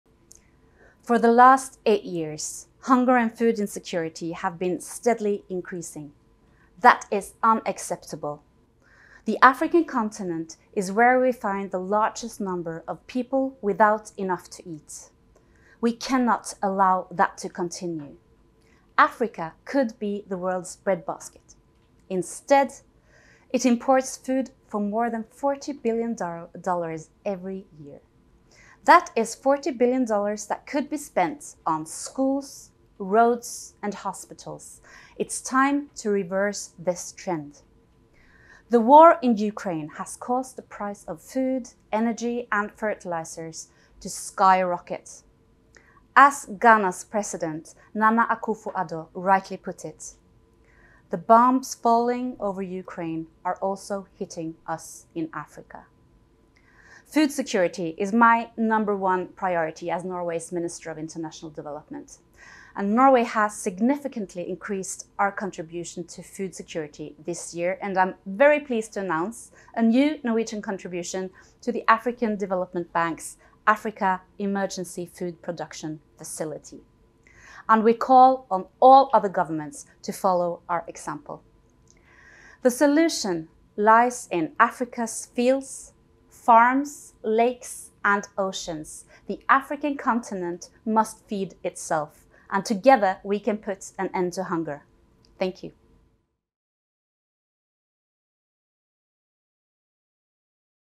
Anne Beathe Tvinnereim Pledges NOK100M to the African Development Bank at Global Citizen Festival
Norway Minister of International Development, Anne Beathe Tvinnereim commits NOK 100 million toward the African Development Bank’s Africa Emergency Food Production Facility on behalf of Norway at Global Citizen Live.